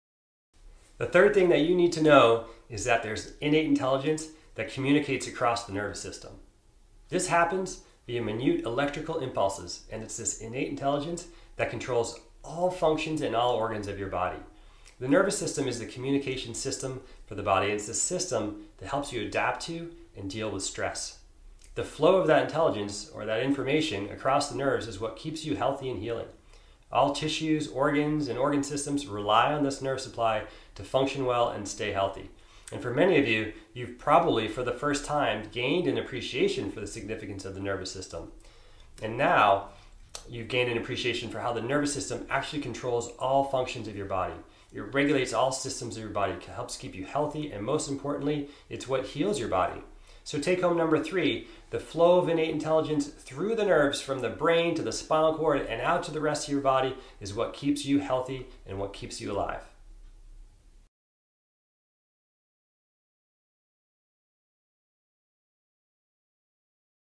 What keeps you alive, healthy, and healing?
There is an innate intelligence that runs every cell, tissue, and organ in your body. It helps you adapt and deal with stress. It regulates all systems of the body, helps keep you health, and most importantly, it is what heals your body. (3 of 12)